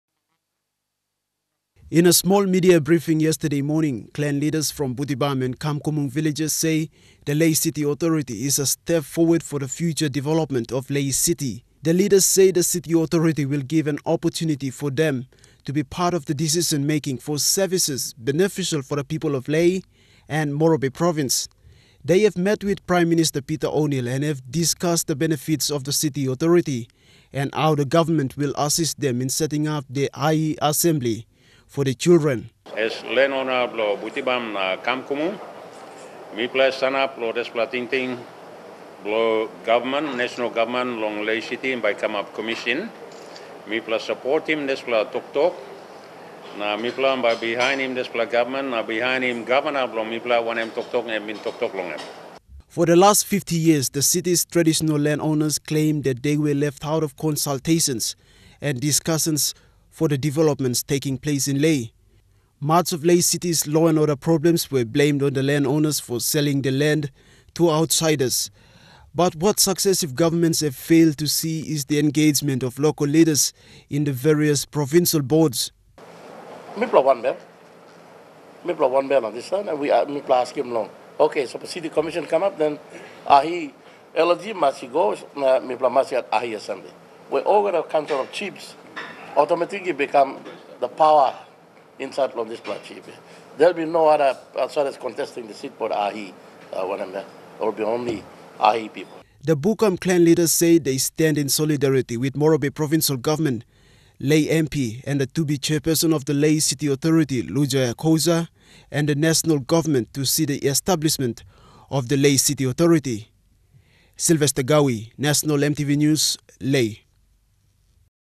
Bukam Leaders Clarify Stance for City Authority
Prime Minister Peter O’Neill has agreed to give them their Ahi Assembly once the Lae City Authority is established - visit us for the latest news...